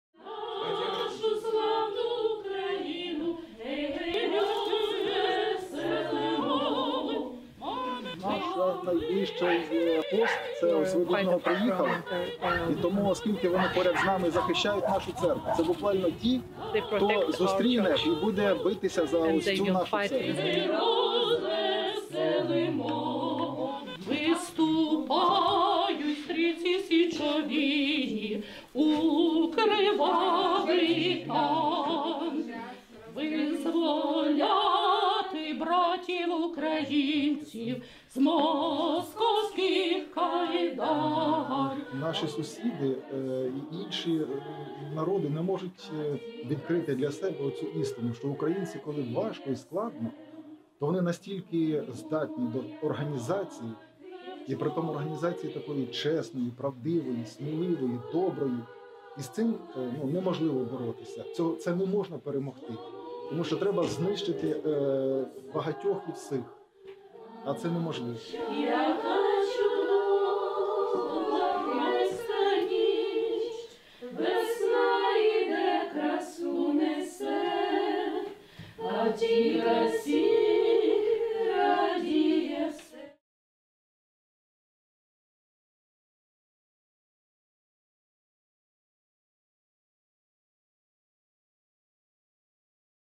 Russian invention: Church Feeds Armed Volunteers In Kyiv, Ukraine
Parishioners at a church on the outskirts of Kyiv prepare food to support volunteers defending the Ukrainian capital. Viktor, a priest with the Orthodox Church of Ukraine, organizes the work with local women.
They prepare traditional meals while singing patriotic Ukrainian songs.